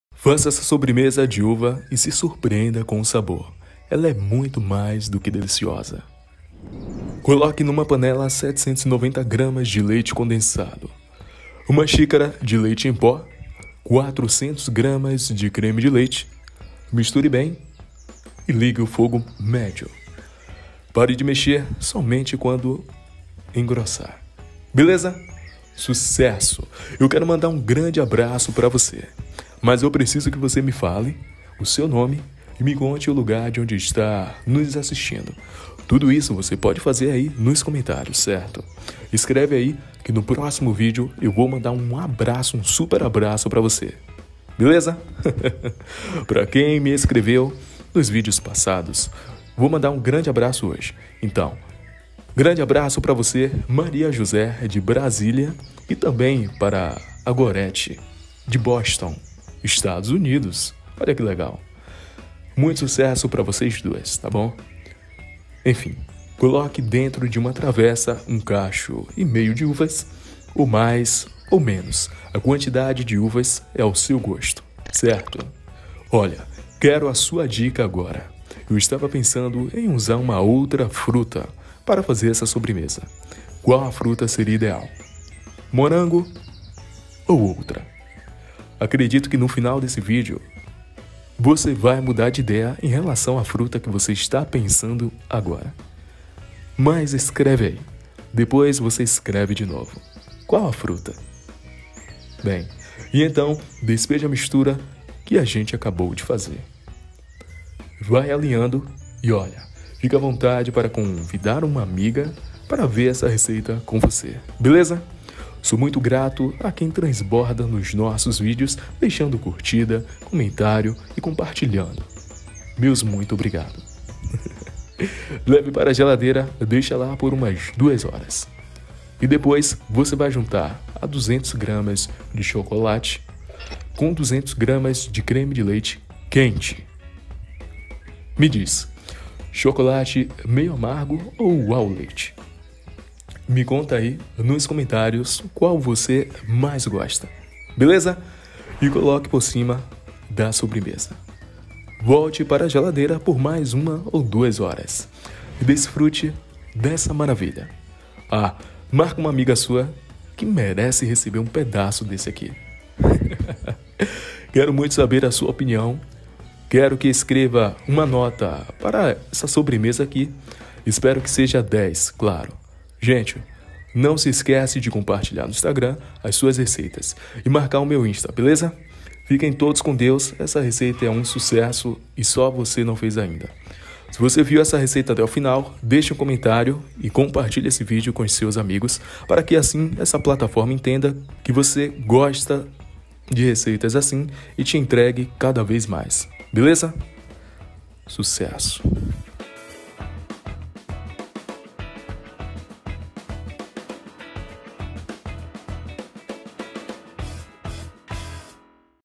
FAÇA ESSA SOBREMESA E SE SURPREENDA COM O SABOR!!! Uma delicia!
Olha os Ingredientes 👇
Na panela:
-2 caixas de leite condensado
-1 xícara de leite em pó
-2 caixas de creme de leite
Mexa bem, e depois ligue em fogo médio... Continue a mexer, e logo vai engrossar, assim estará pronto!
Na travessa:
-uvas à gosto para forrar o fundo da travessa (utilizei um cacho e meio)
-coloque a mistura de creme branco por cima!
+ Creme da finalização:
-200 g de chocolate
-1 caixa de creme de leite bem quente
Misture os dois, derreta o chocolate... Coloque por cima do creme branco para finalizar a sobremesa!
Leve para gelar e estará pronto!
Se Você leu os ingredientes, deixa um SUCESSO aqui em baixo, pode ser? 👇